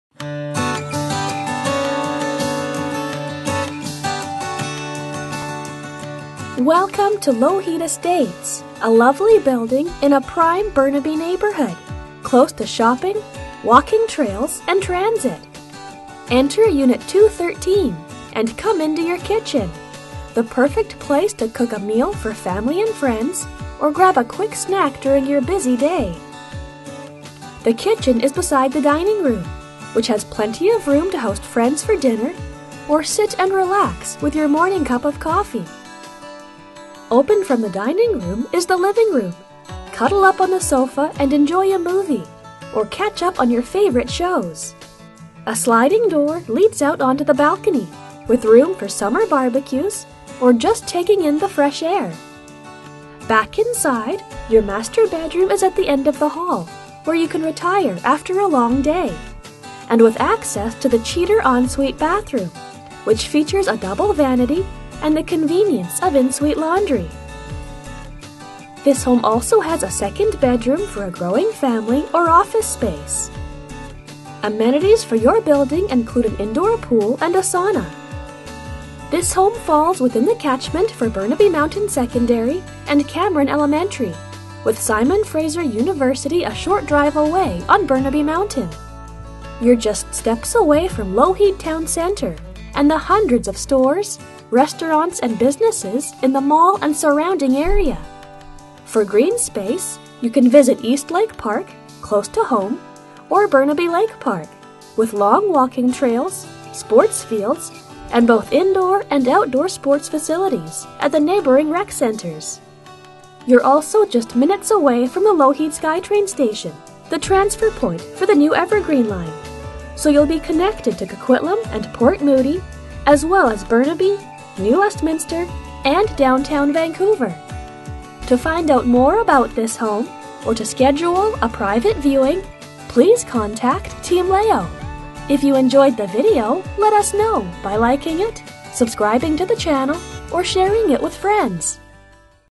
Unit 213 3921 Carrigan Court, Burnaby
LOCATION! LOCATION! Well maintained 2 bedroom and 1 bath complete with in suite laundry. Spacious and practical floorplan! Living room with sliders out to covered balcony, updated kitchen with tile backsplash, cabinetry and counters. Two ample sized bedrooms! Lots of in suite storage. Excellent amenities! Indoor pool, sauna, hot tub and recreation room. Fantastic location! Walk to all shopping and services of North Road and Lougheed Mall. Close to all levels of schools. Easy access to Hwy 1. One block to skytrain for quick commute downtown or evergreen line extension to Coquitlam centre. Heat and hot water included in maintenance fee! Don't miss this opportunity!